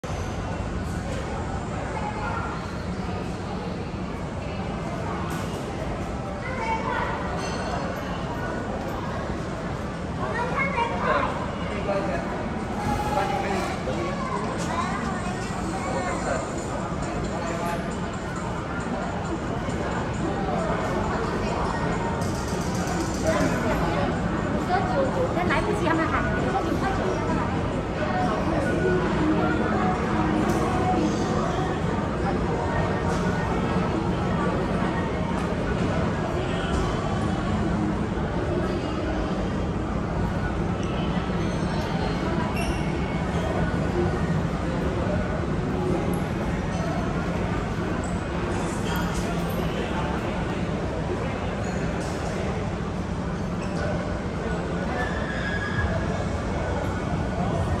TRAVEL GUIDE for Level 7 Pavilion Kuala Lumpur city | Serai Rocku Yakiniku Al Halabi 2022 #9
Must Watch! Amazing modern and luxury "little Dubai" shopping haven in Kuala Lumpur. If you are planning to visit Kuala Lumpur, Pavilion is a must-visit in your itinerary.

DO NOT REPRODUCE USING ANY OF OUR VIDEOS.